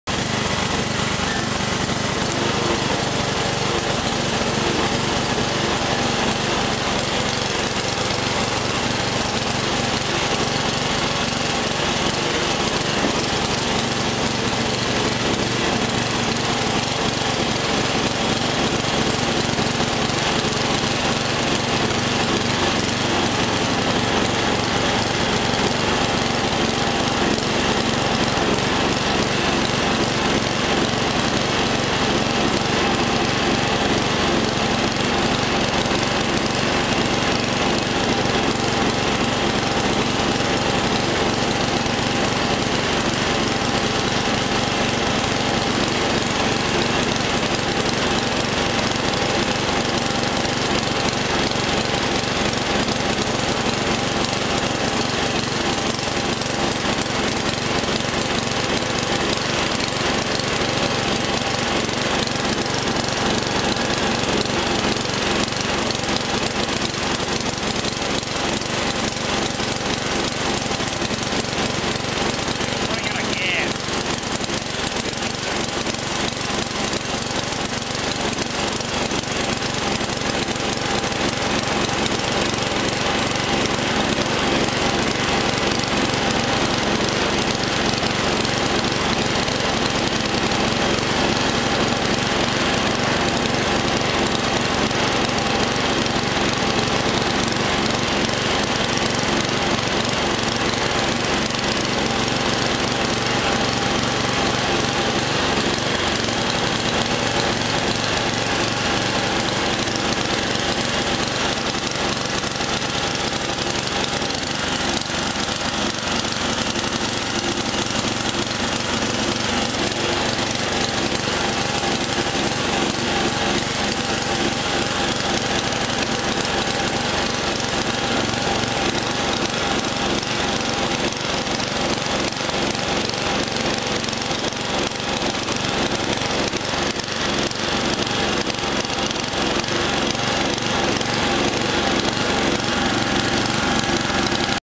Craftsman LT11/36 Running
It runs!
Still needs to be tuned up a little bit, but it now runs, drives, and cuts!

This is a quick tour around the house, from the back yard, around the side, around the trees, and back into the back yard.